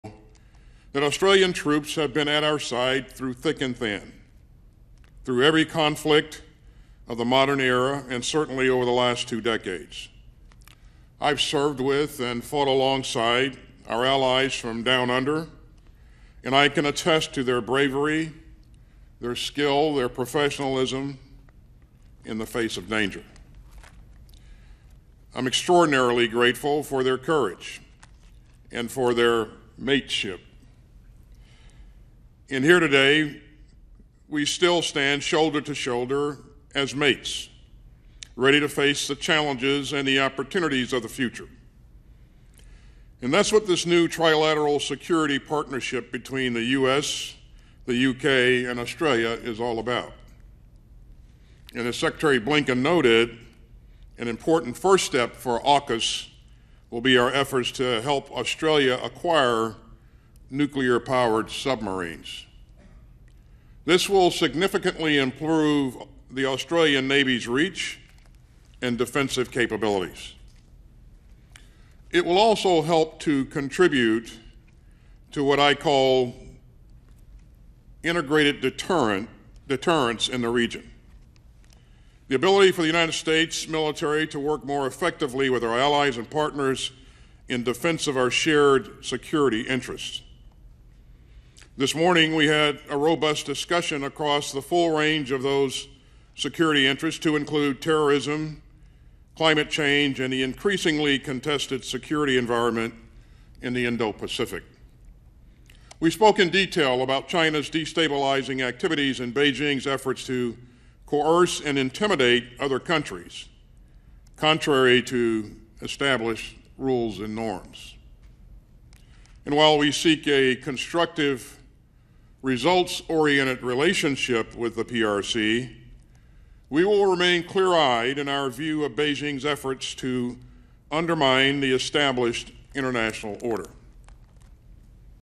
Sec. Austin: China UNDERMINING international order
In a public address Thursday, Secretary of Defense Austin said China is actively trying to undermine the international order.